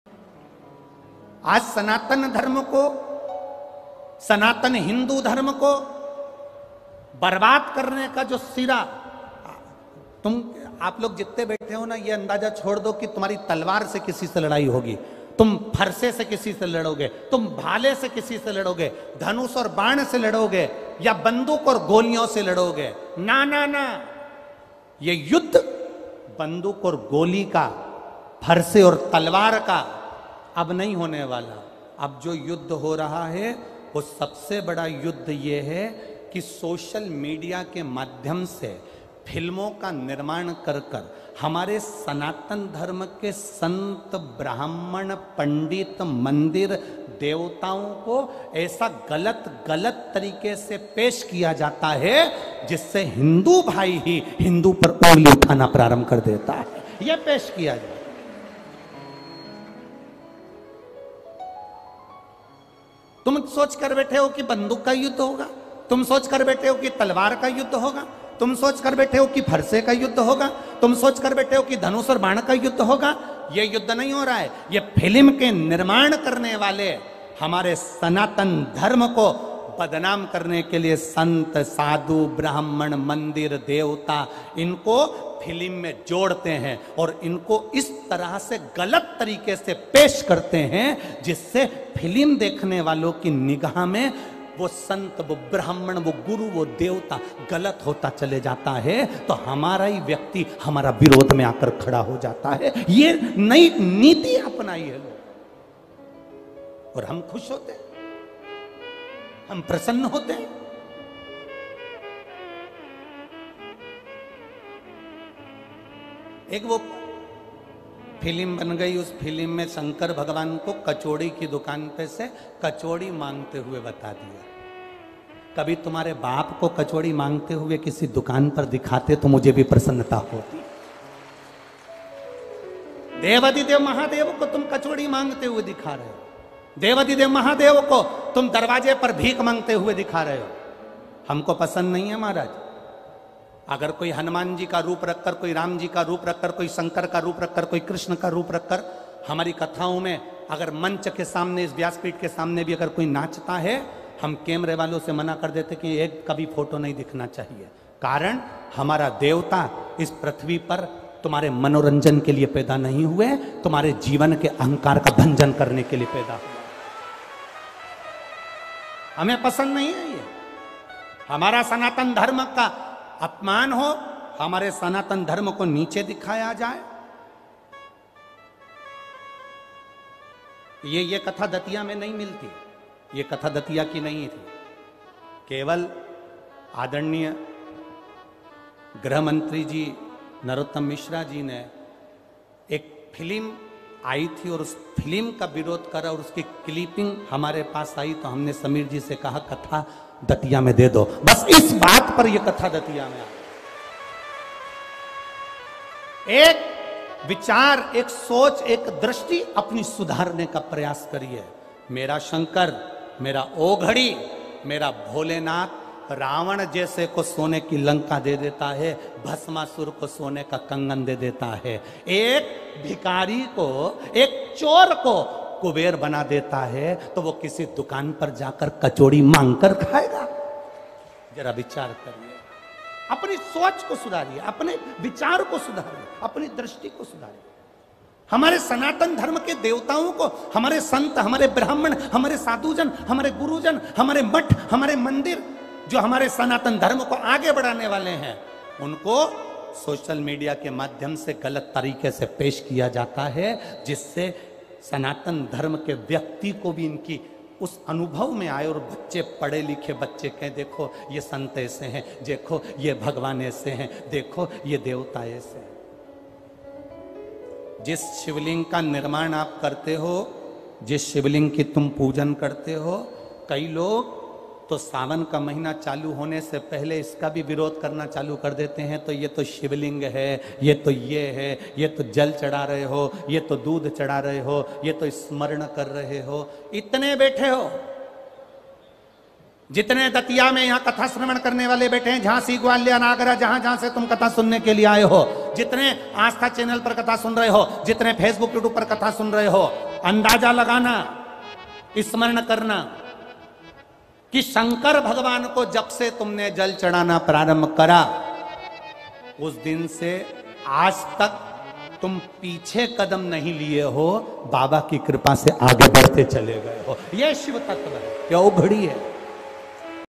सनातन धर्म को आहत करने वाली फिल्म को लेकर महाराज जी जाने क्या हैं - Pandit Pradeep Ji Mishra
सनातन धर्म  को आहत करने वाली फिल्म को लेकर महाराज जी जाने क्या हैं - Pandit Pradeep Ji Mishra Sehore Wale